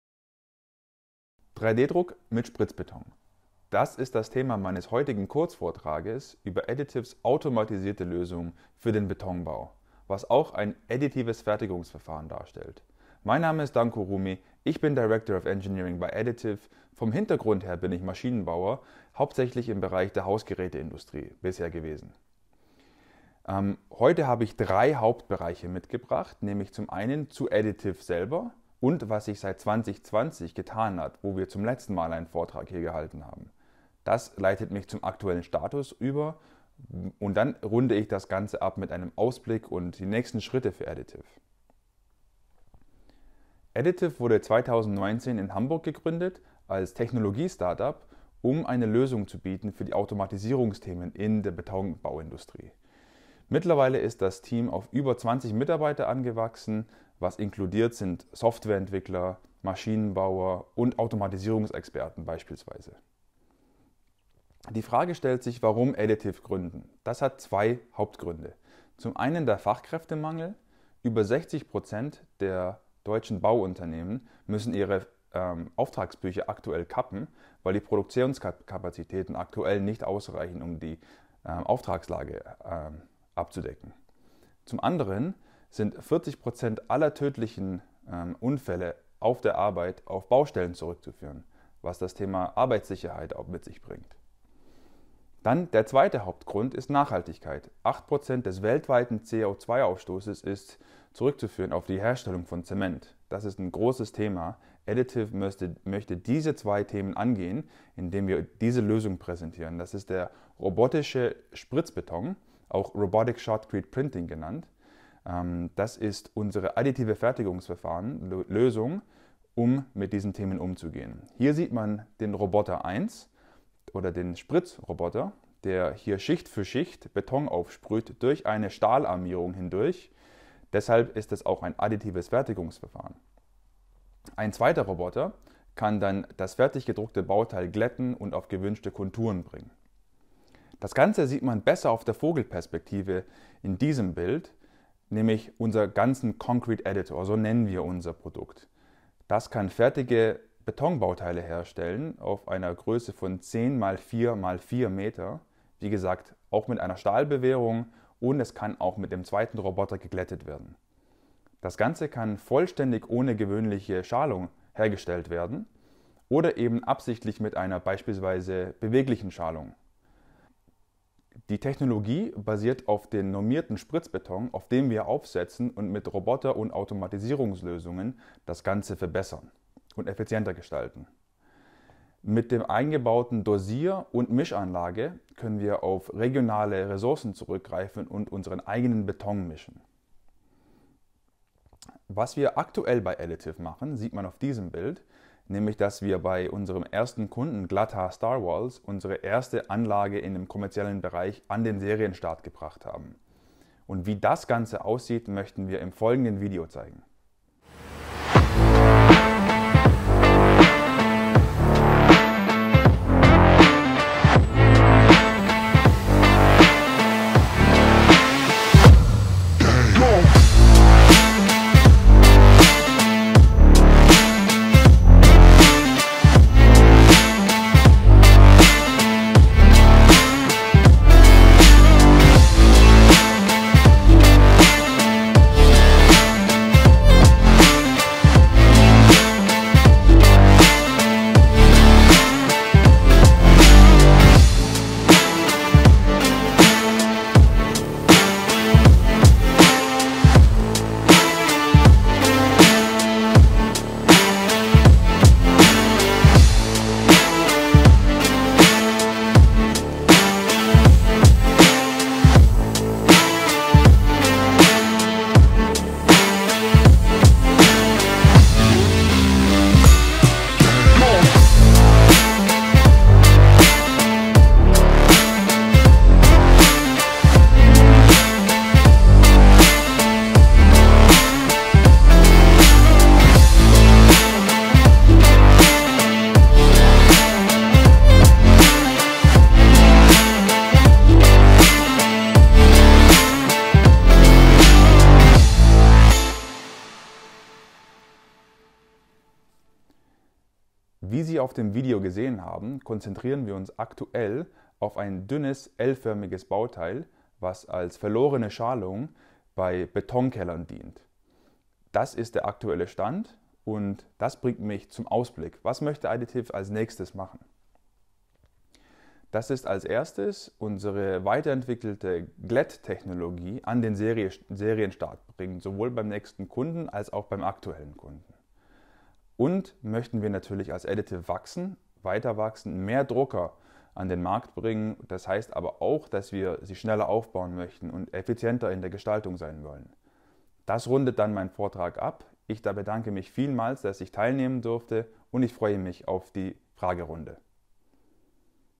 3D-Druck mit Spritzbeton - Danko RUMI
Im Jahr 2022 lieferte das Technologie-Startup Aeditive den ersten Spritzbetonroboter aus, der nun im kommerziellen Einsatz ist. Danko Rumi zeigt in seinem Vortrag auf dem 4. Deutschen Industrieseminar die wesentlichen Entwicklungsschritte der letzten zwei Jahre.